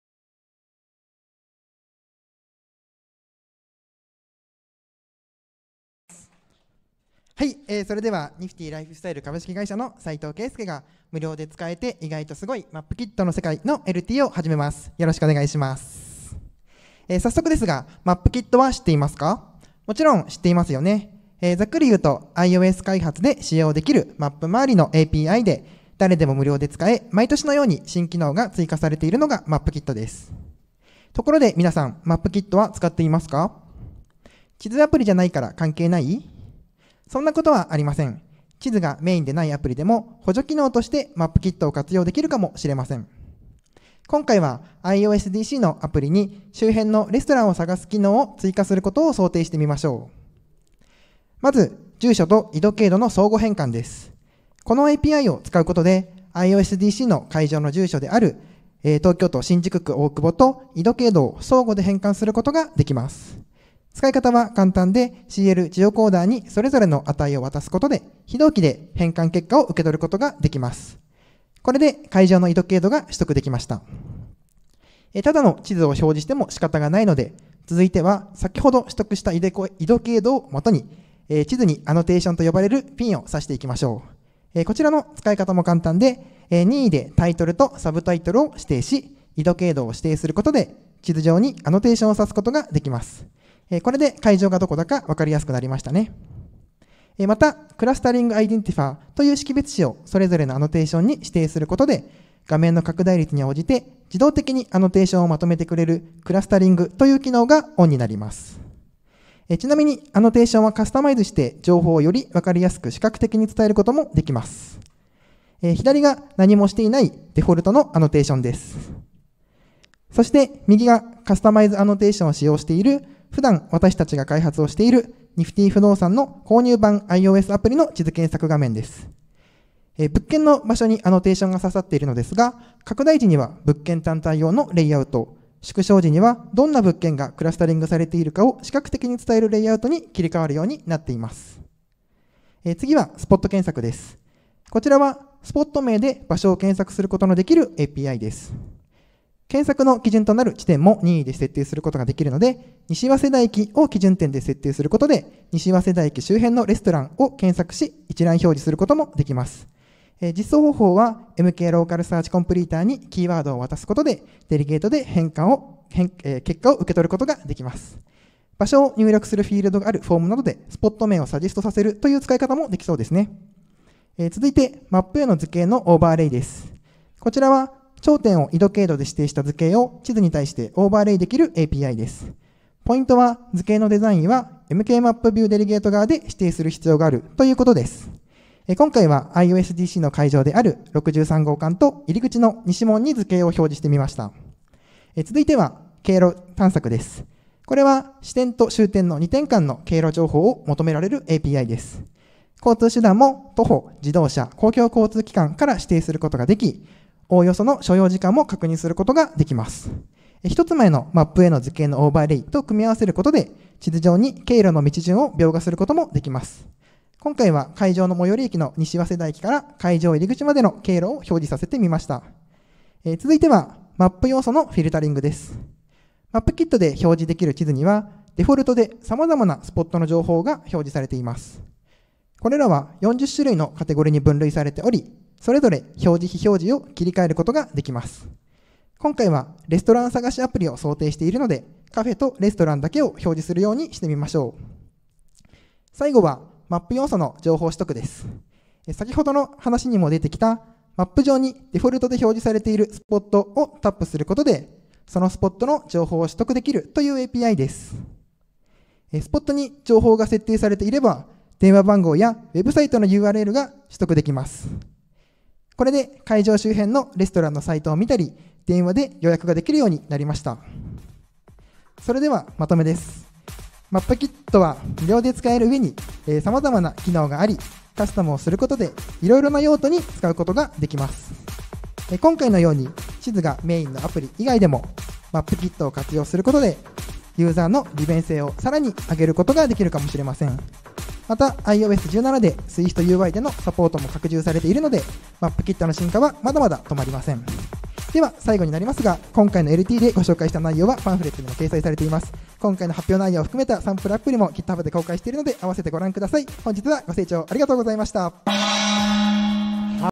iOSDC Japan 2023: 無料で使えて意外と凄いMapKitの世界 / Keisuke Saito
iOSDC Japan 2023 のセッション動画です。

2023/09/02(5分)
スピーカー: Keisuke Saito ( @saikei718 )
タイトル: 無料で使えて意外と凄いMapKitの世界

Apple標準フレームワークでお馴染み「MapKit」ですが、
「ただ地図にピンを立てるだけ」ではなく様々な機能がいくつもあることを、みなさん知っていますか？

実はMapKitには毎年新機能が追加され、年々出来ることも増えてきています。

- スポット名での検索＆サジェスト
- A地点からB地点への経路探索
- 緯度経度と住所の相互変換（ジオコーディング）
- ピンのカスタマイズやクラスタリング
- 地図上のスポットアイコン（POI）のフィルタリング
- マップ上への図形の描画

など、このLTでは実際にプロダクトに導入した経験も踏まえ、
MapKitを使い無料で実現できる様々なカスタマイズや機能についてご紹介します！